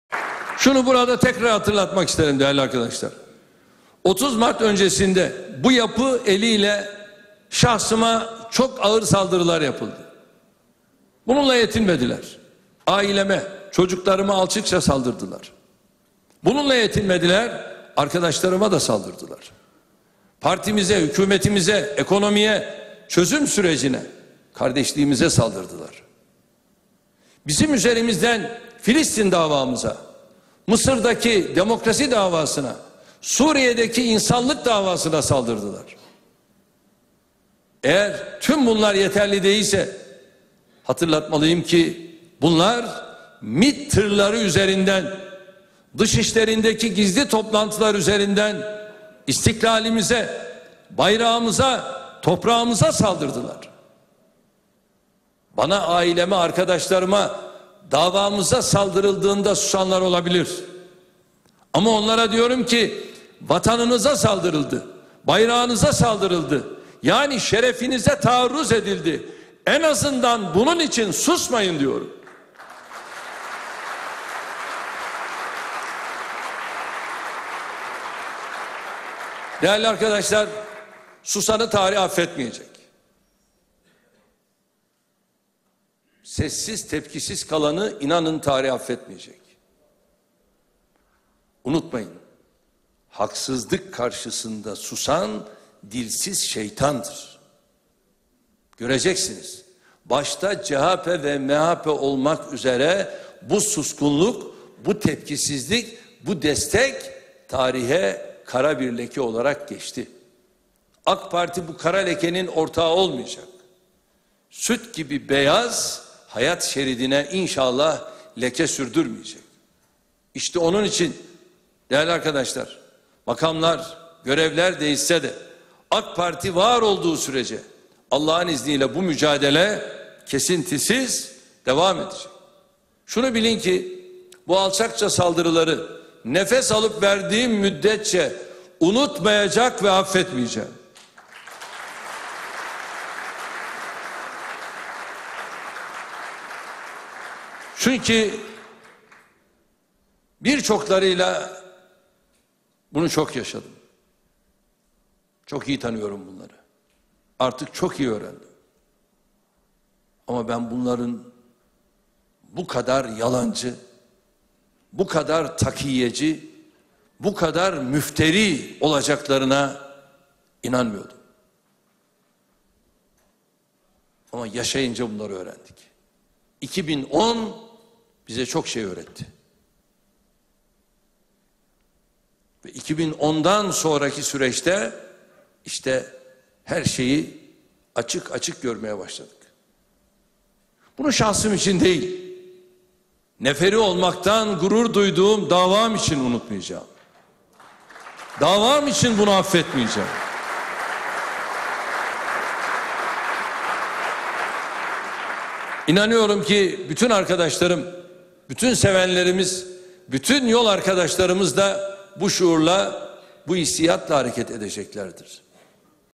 Başbakan Erdoğan: Davam için asla affetmeyeceğim
Başbakan Recep Tayyip Erdoğan partisinin Afyon kampının kapanış konuşmasında yerel seçim öncesi olaylara ilişkin önemli açıklamalarda bulundu